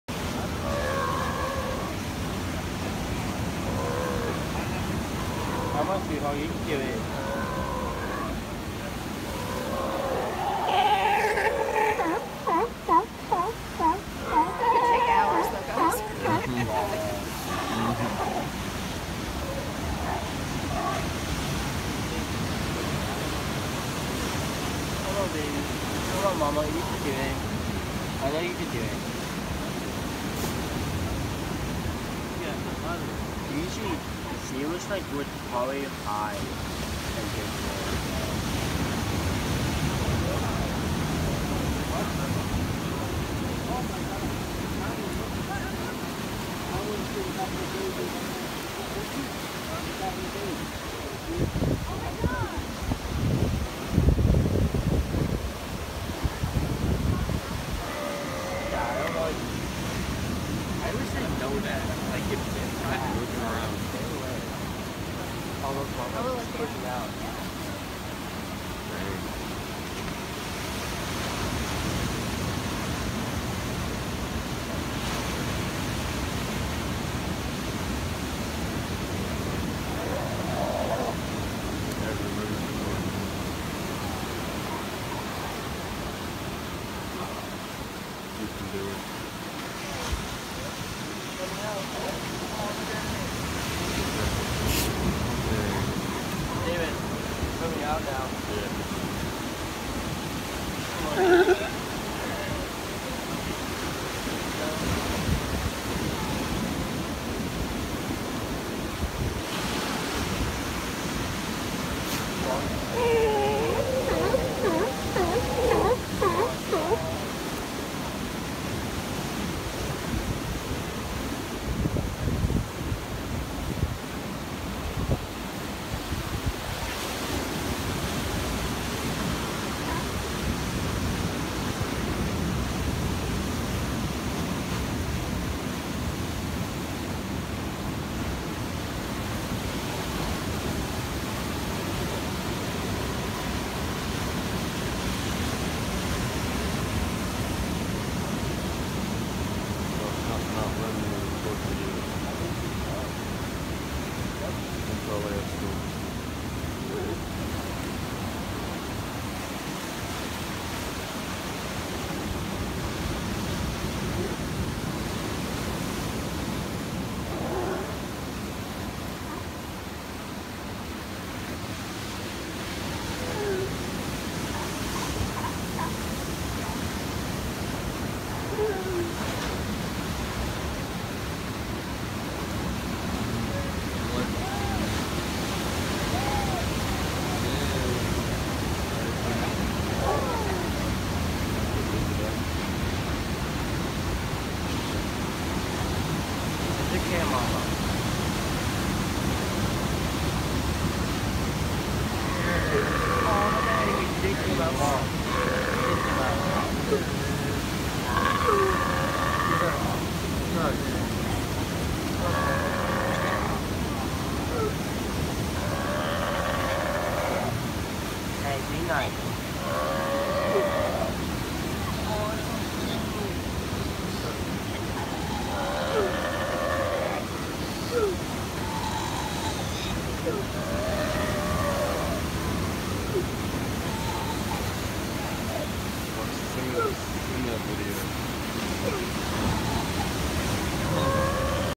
Sea Lion Gives Birth! Must See! Raw Footage!
Sea Lion / Seal gives birth to baby! Hard, rare sighting! How crazy! Orange street San Diego CA